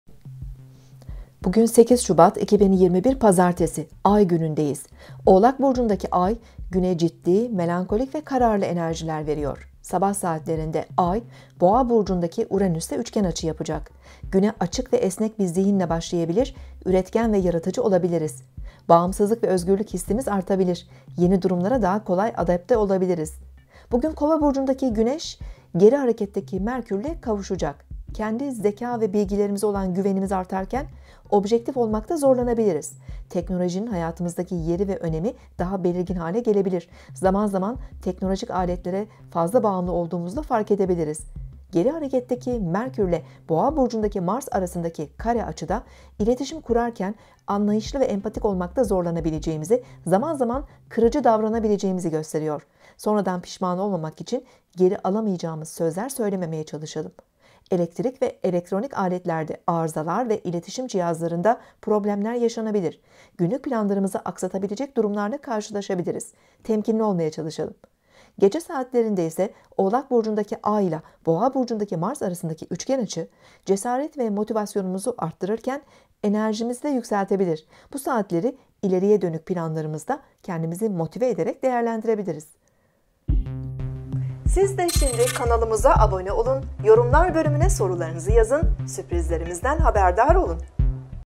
Günlük Burç Yorumları 8 Şubat 2021 Pazartesi Astroloji burç
Günlük Burç Yorumu 8 şubat 2021 pazartesi (Tüm burçlar)
Bugün 8 şubat 2021 pazartesi, günlük burçlar ve astroloji yorumu (günlük burç yorumları)